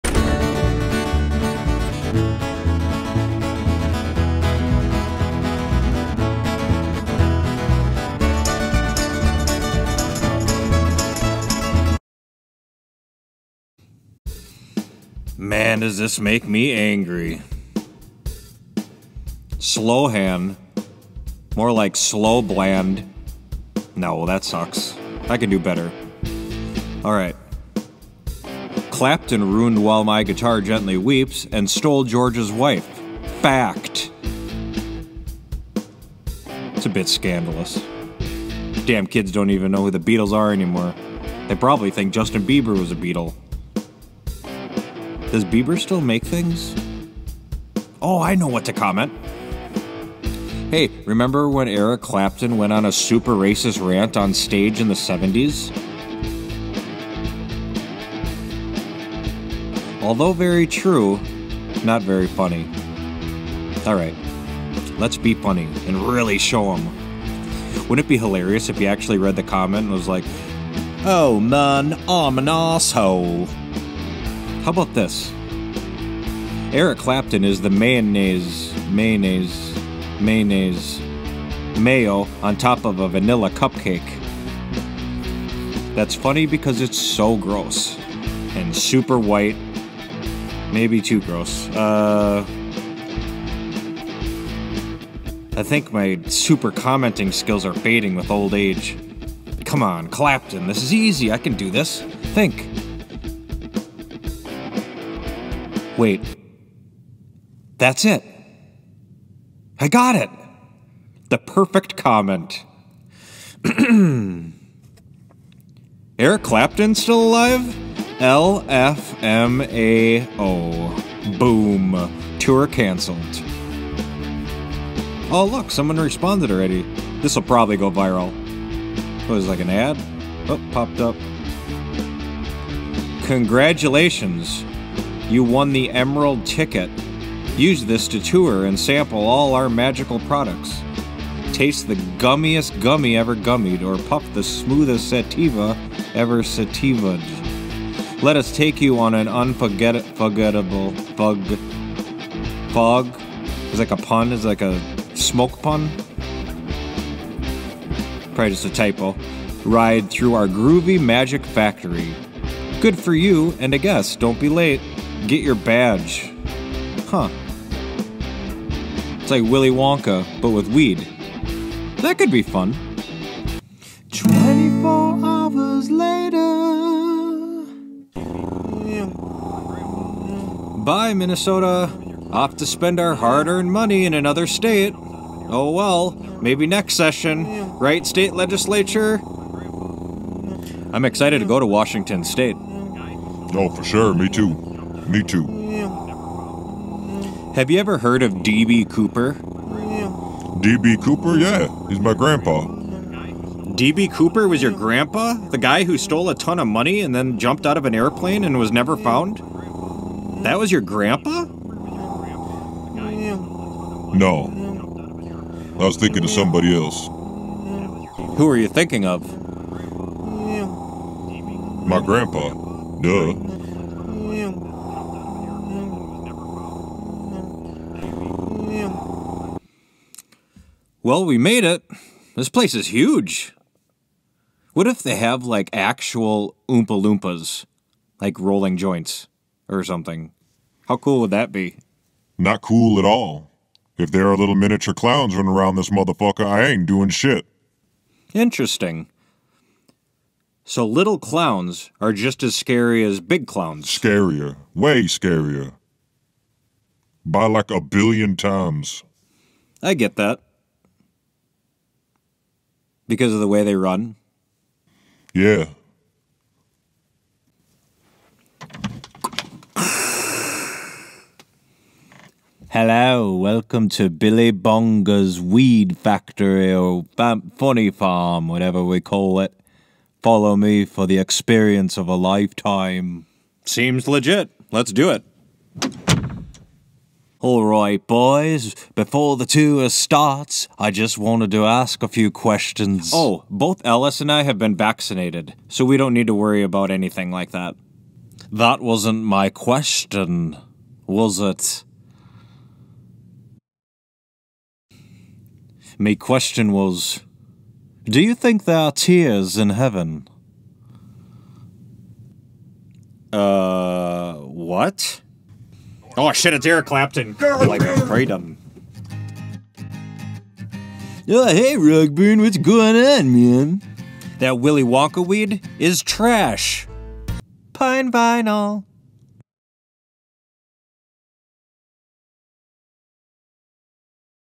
Billy Bonga And The Funny Factory By Pine Vinyl
After and unexpected invite things turn sweet in this send up of Charlie and the Chocolate Factory!